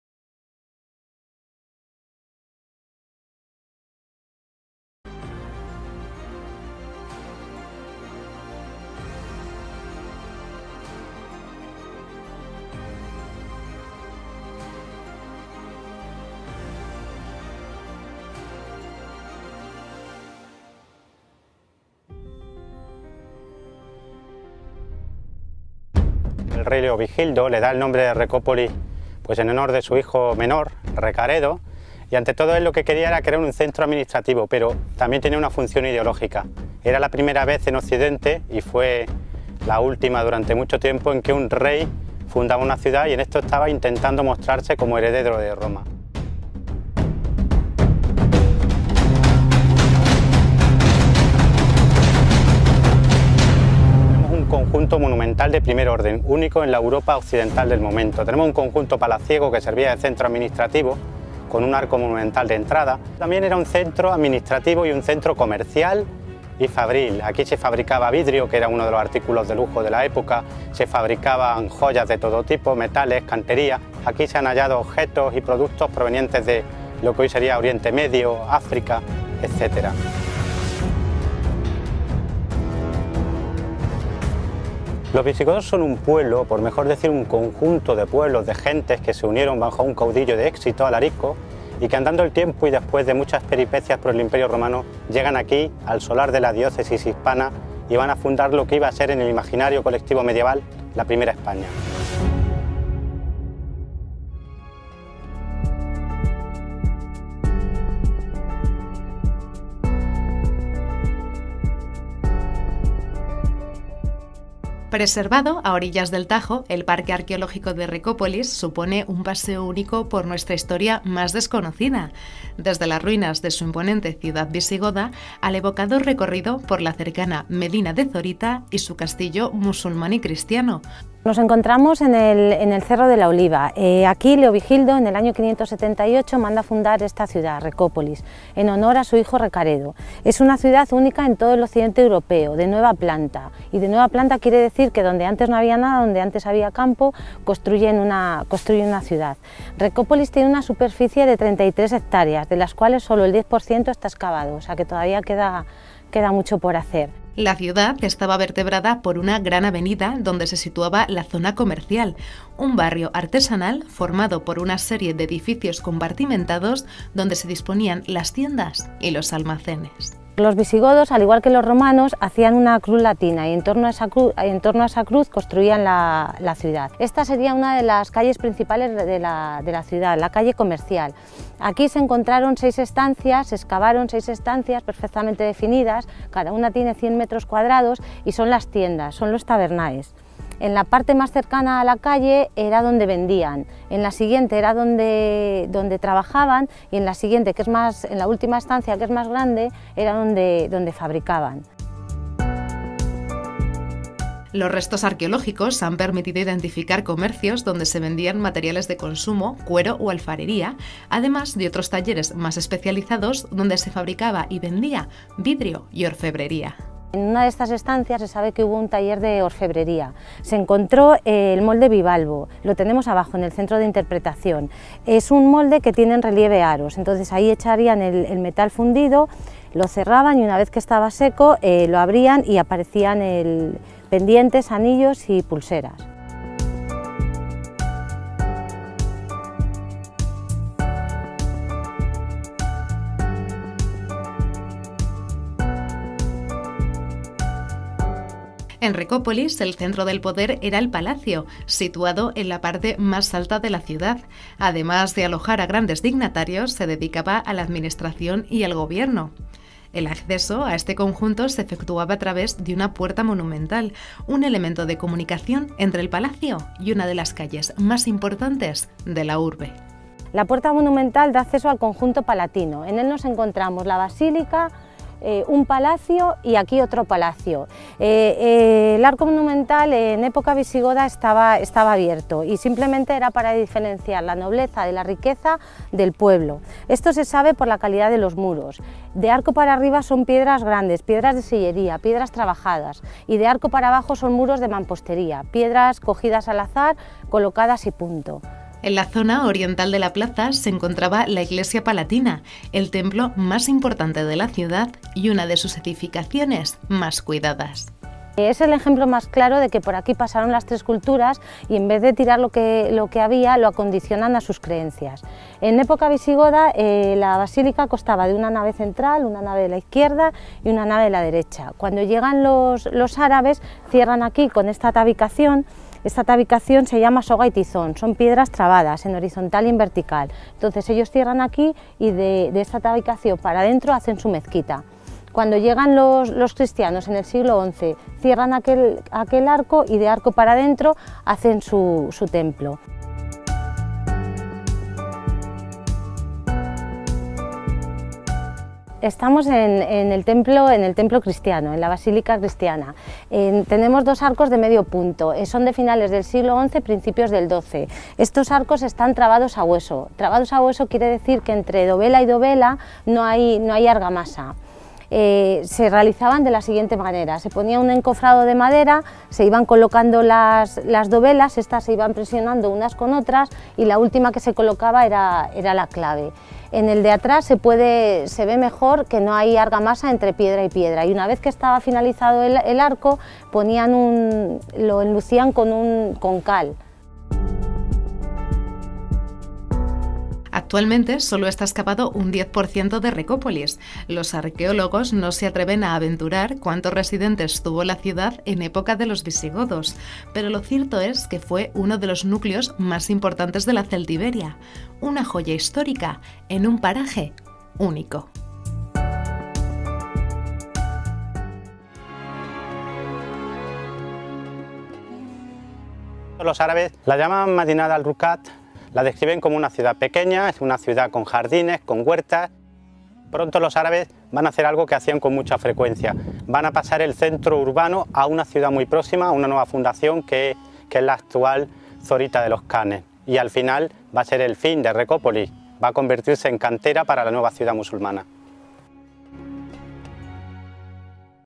Recópolis, ciudad fundada por los visigodos en Guadalajara
Recópolis, en la provincia de Guadalajara, mandada construir por el monarca visigodo Leovigildo en el año 578, es la única ciudad fundada por iniciativa regia en los inicios de la Alta Edad Media en Europa. Fue capital de una provincia denominada Celtiberia, de límites imprecisos, situada al Este de Toledo, la capital del reino.